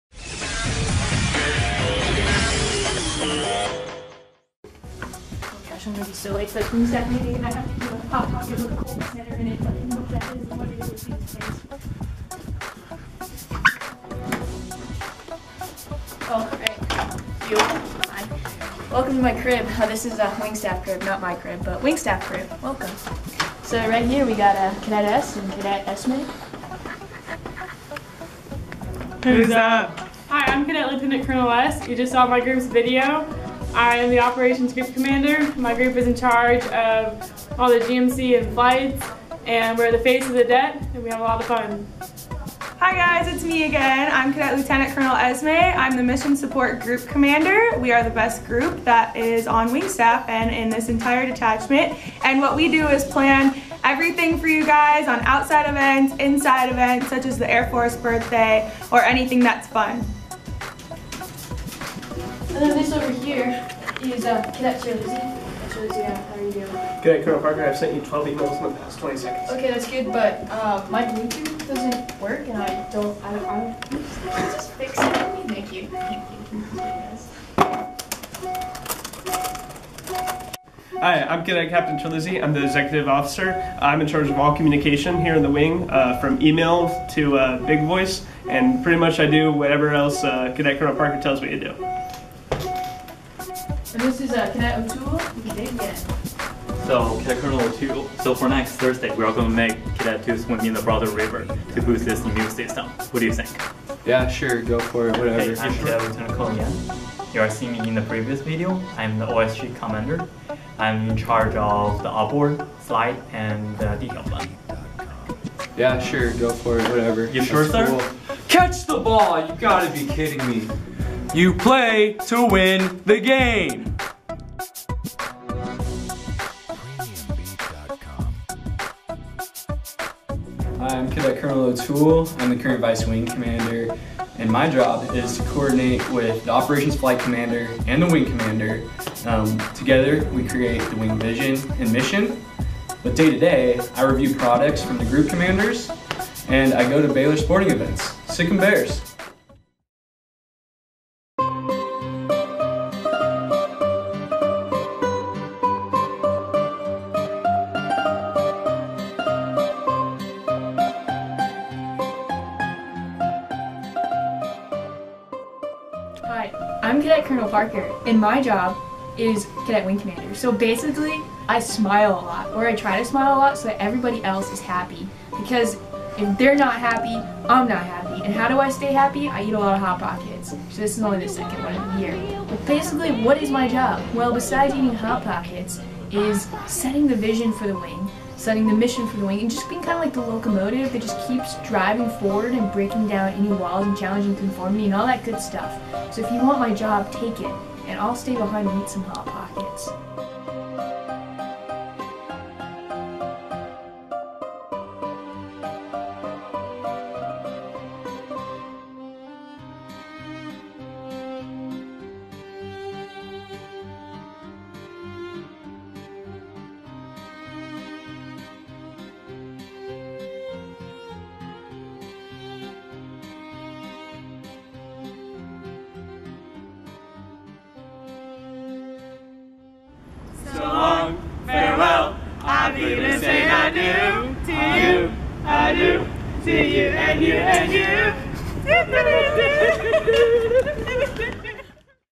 Wing Staff Cribs - Fall 2018
AFROTC Detachment 810, Baylor University - Wing Staff's "MTV Cribs" Video Rendition

Created for GMC Takeover Week - Fall 2018

Video Created by: Connor Paw